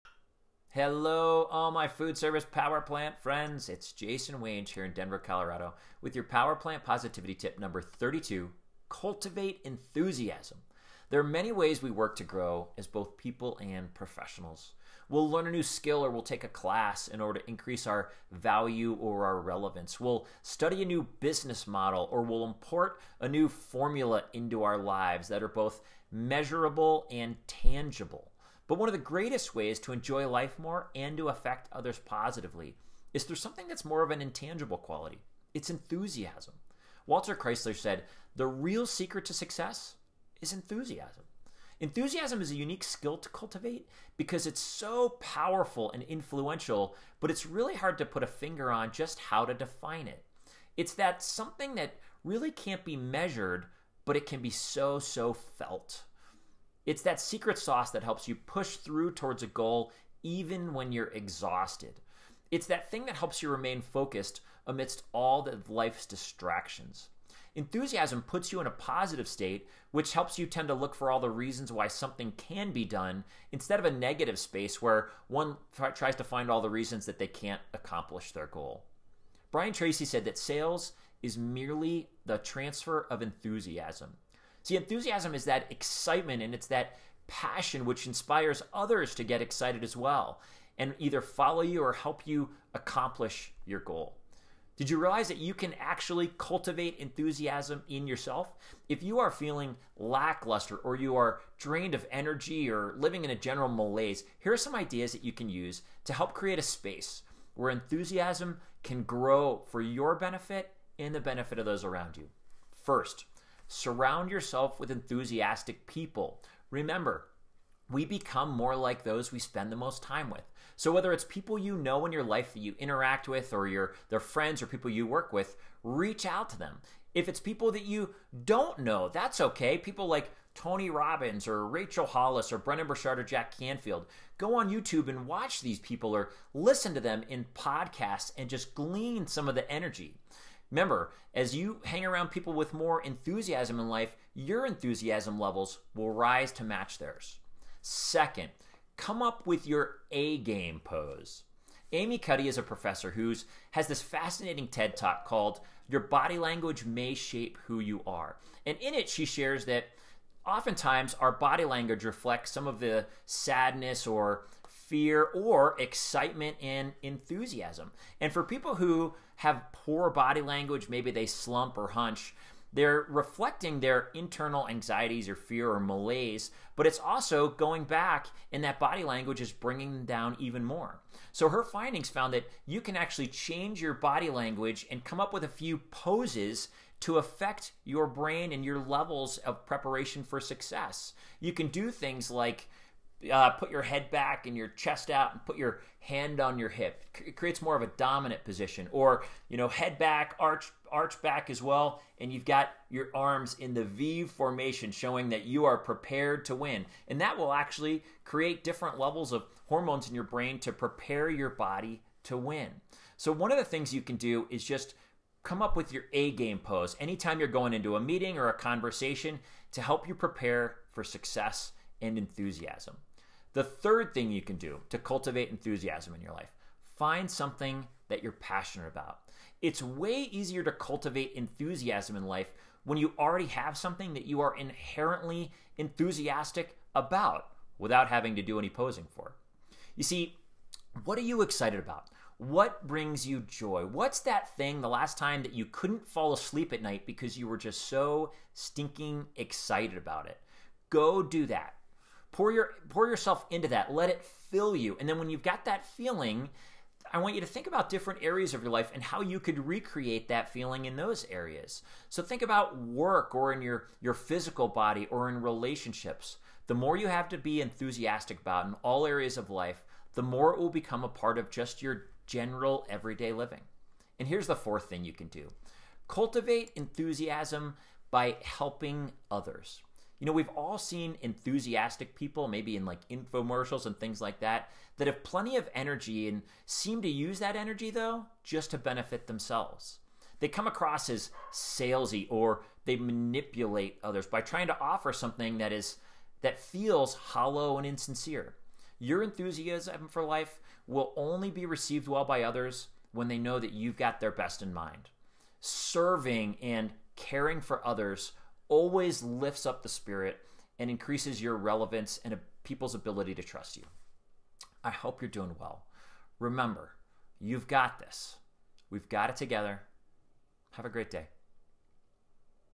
Positivity Tip 32: Cultivate Enthusiasm
We are often working to grow as people and professionals in ways that are measurable.  But one of the greatest attributes of positive and successful professionals is more of an intangible...Enthusiasm.

Enthusiasm is what keeps you working your tail off when you are exhausted.  It's that thing that helps you stay focus amidst life's distractions.  And it's the quality that helps get others to want to follow you or help support your vision.

Here are 4 Tips to Cultivate Enthusiasm in your Life:
1. Surround Yourself with Enthusiastic People
2. Come up with your A-Game Pose!
3. Find something to be passionate about and grow from there
4. Create an Enthusiasm for helping others